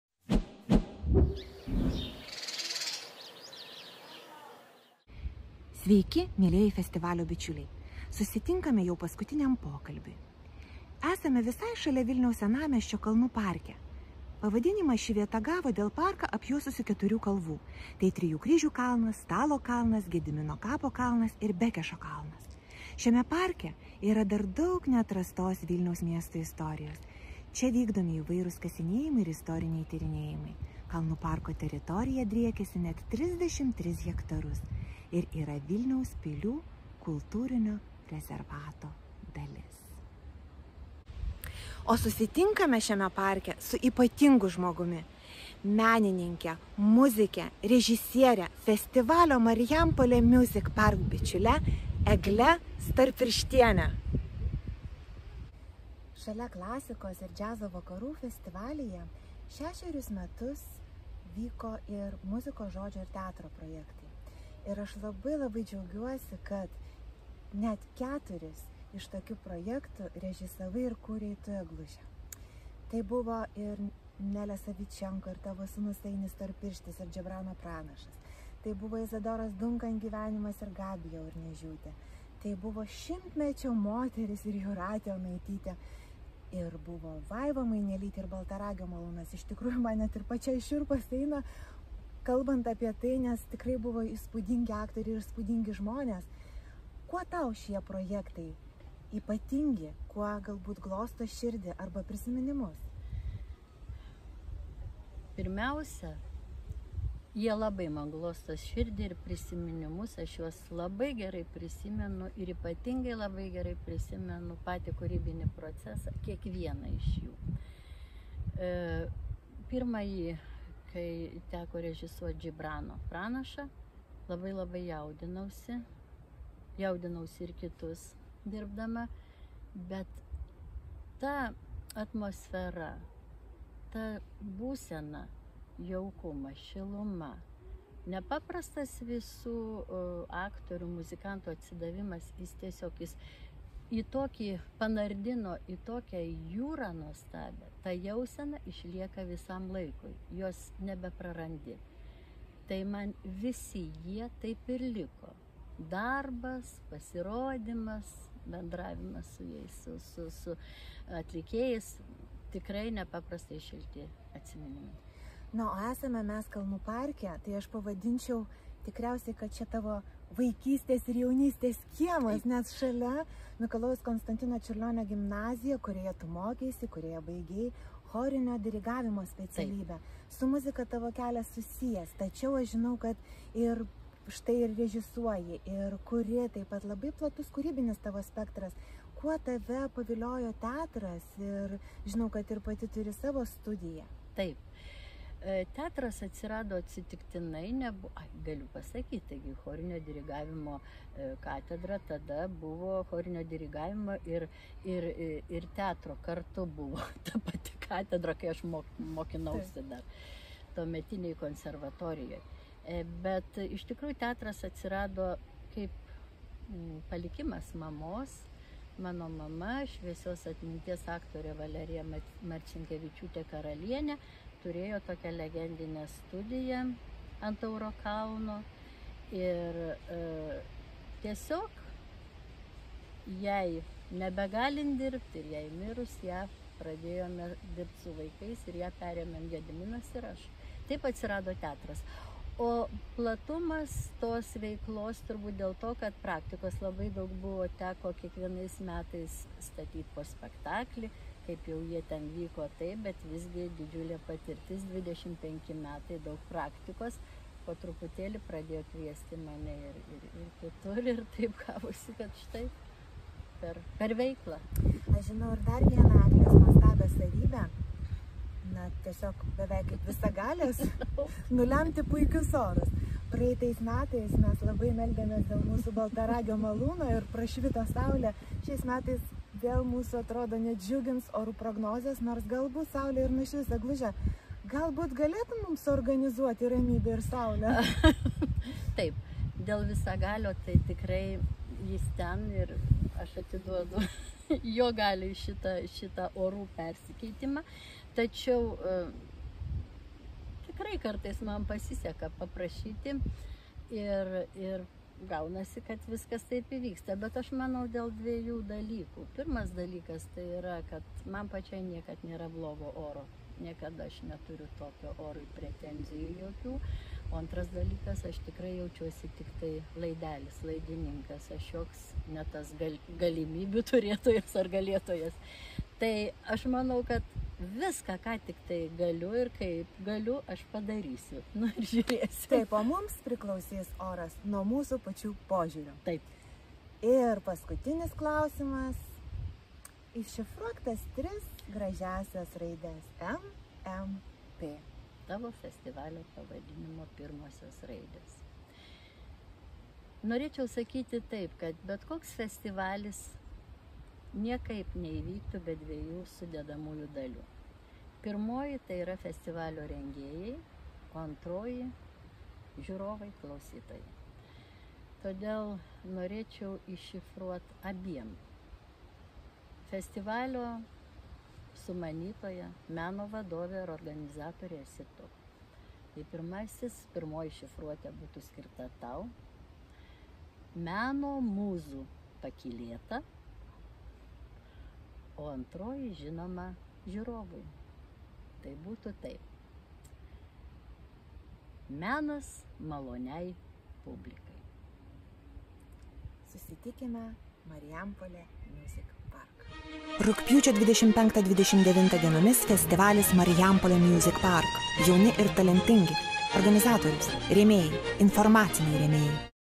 Festivalis "Marijampolė Music Park"/Pokalbiai parkuose/Eglė Storpirštienė
Festivalis "Marijampolė Music Park"  demonstruoja mini interviu ciklą su festivalio atlikėjais, kūrėjais, žiūrovais ir įdomiais žmonėmis. "Pokalbiai parkuose" - taip pavadintas šis interviu ciklas nufilmuotas vis kitame Vilniaus miesto parke, skvere, aikštėje.